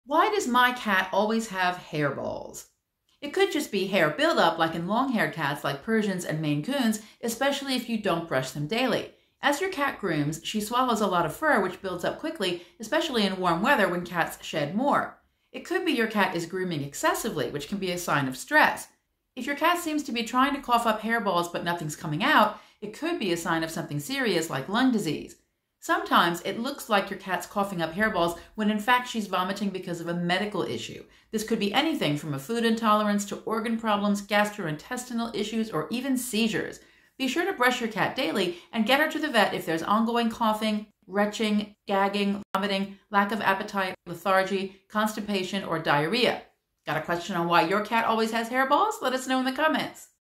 WHY Does my Cat ALWAYS Have Hairballs?
Today Barbara will be explaining why your cat may be constantly suffering with hairballs and how to help them!